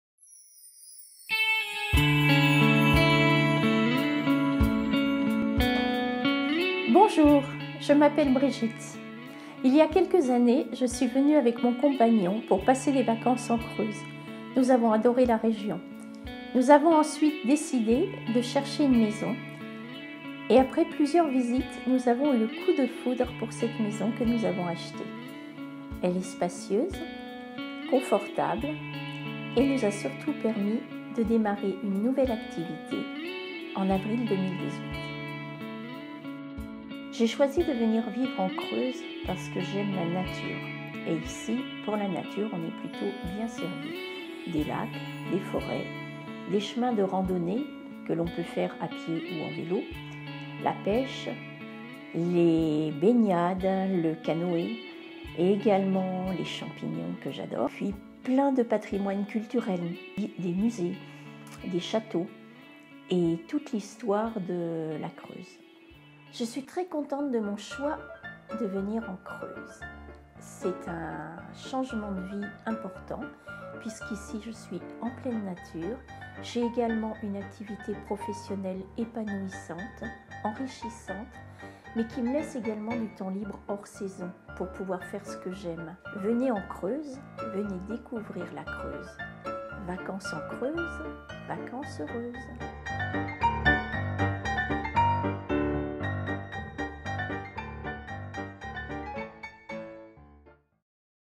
Pourquoi La Creuse ?
Interview d'une femme qui s'est reconvertie, dans la Creuse
Désormais elle tient Les Chambres du Lac à Saint Marc à Loubaud, chambres et tables d'hôtes